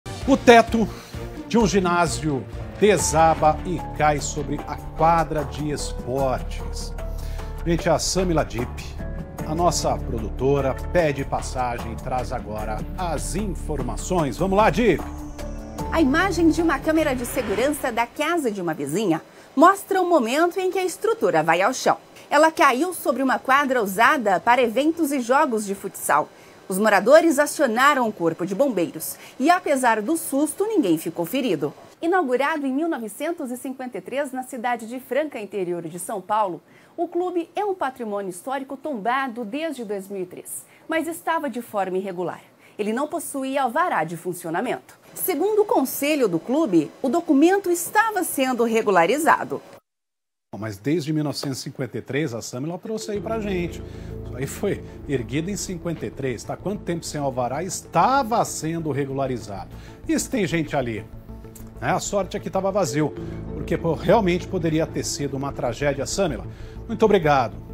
Tá no Ar Campinas - Teto de ginásio desabada e cai sobre quadra de esportes
Segundo informações, o local não possuia alvará